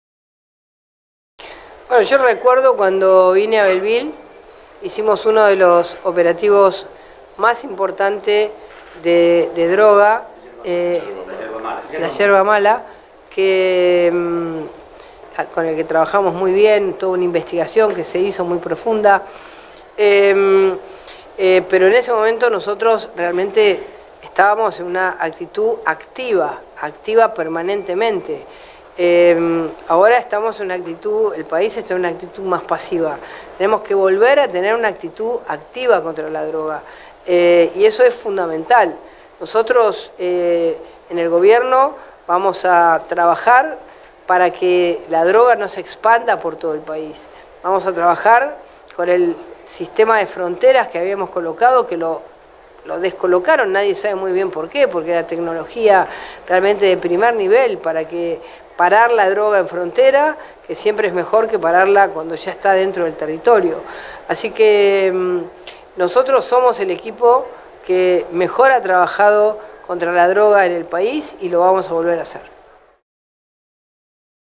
PATRICIA BULLRICH ESTUVO DE PASO POR NUESTRA CIUDAD (2º)
La dirigente nacional resaltó que uno de los grandes desafíos que tendrá el próximo gobierno es ponerle límites al flagelo de la droga.